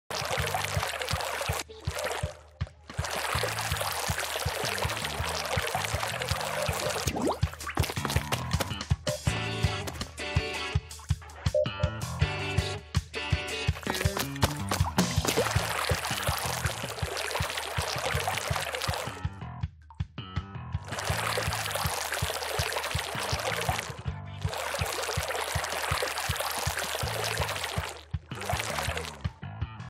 Test area ink-out
Basically, it's just a not-so-impressive video of the whole test room covered with ink.